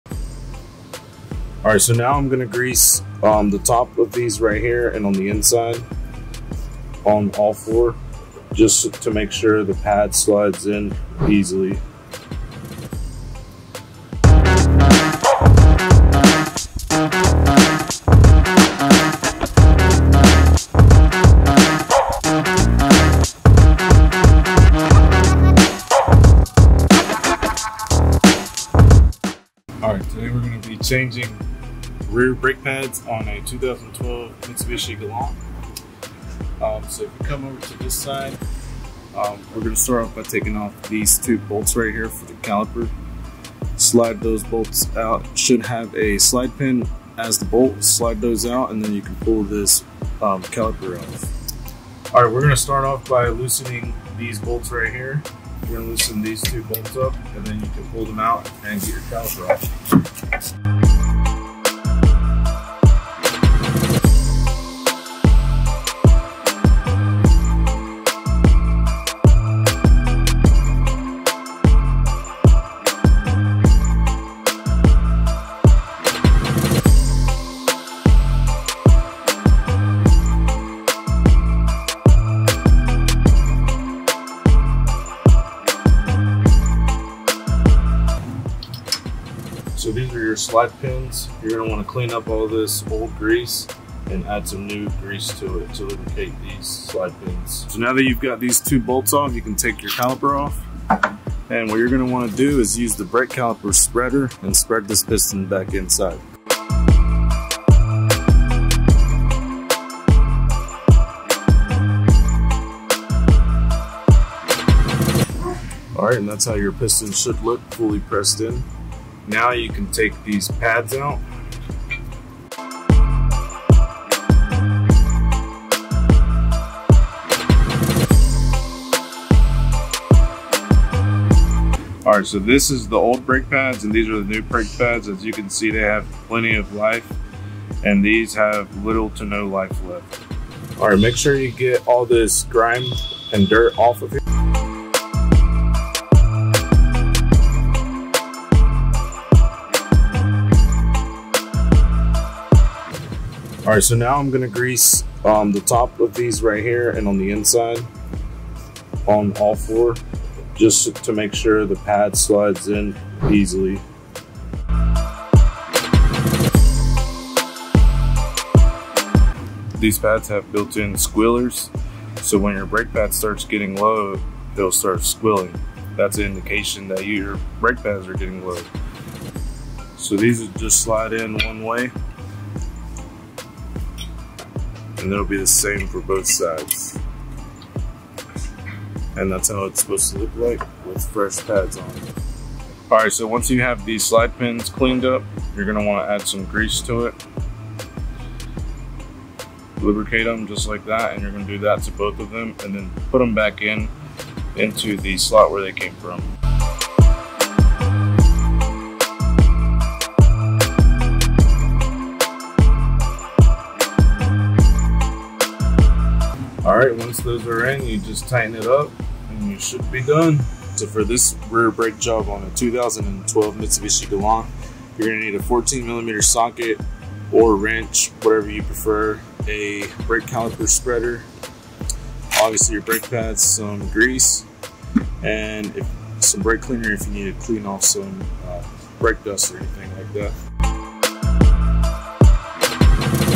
How to change brake pads | COMPLETE TUTORIAL
Full guide on how to replace your break pads!

Leave a comment down below if you would like to know how to do a specific fix on your car, and sure to like, share, and subscribe if you like the content!